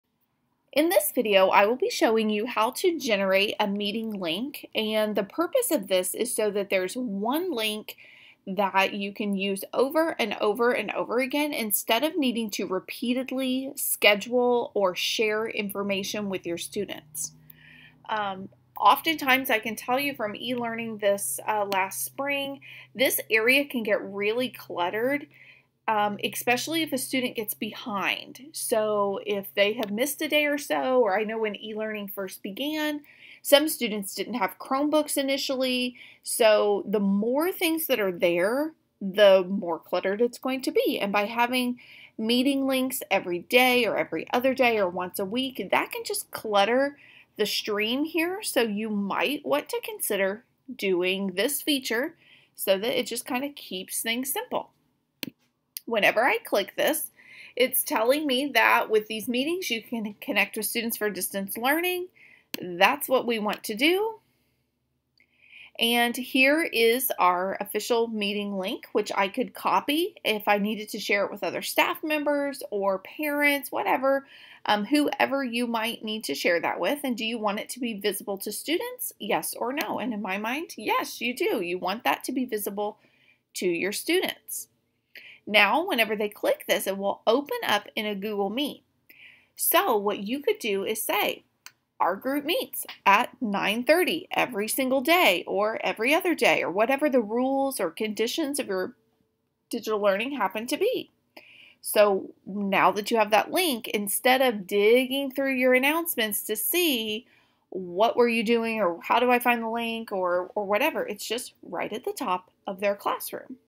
How to Generate a Permanent Meeting Link in Google Classroom
To save time and reduce clutter in your Google Classroom Stream, you can create a permanent meeting link. In this video, I'll show you how to create a meeting link.